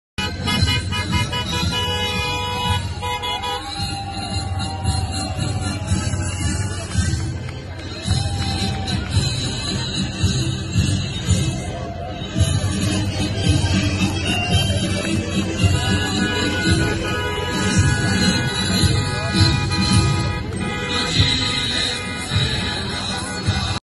Movimientos partidarios de Pinochet, se reunieron en la Escuela Milita para conmemorar el 11Sep
En un nuevo aniversario del golpe militar comandado por Augusto Pinochet que derrocó al presidente Allende, distintas organizaciones y movimientos partidarios de Pinochet, se reunieron en el frente de la Escuela Milita para realizar un acto conmemorativo por el 11DeSeptiembre